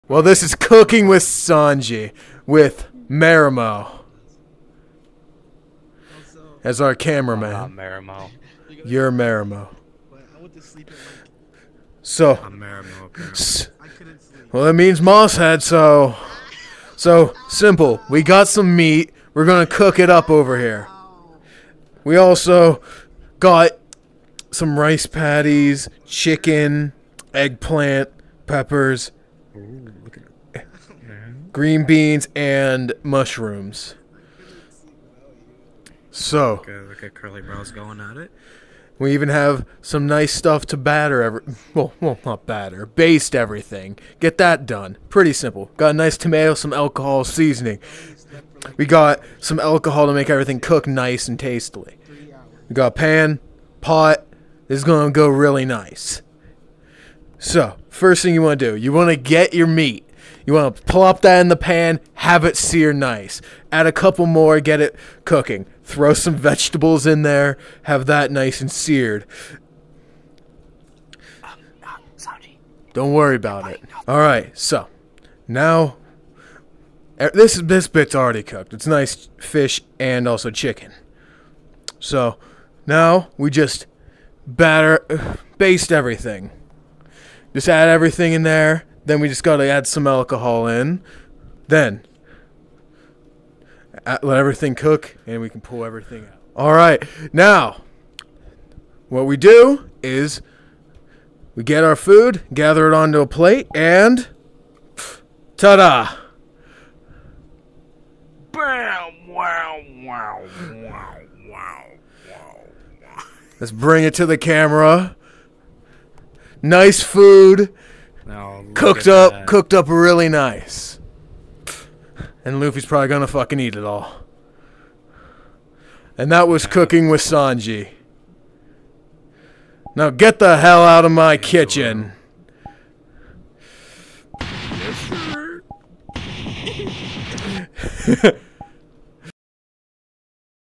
Cooking with Sanji and Mosshead
Sanji and mosshead cook up a nice meal! (sorry about the water mark im still new to editing) if you listen closely you can here Luffy getting beat by Nami somewhere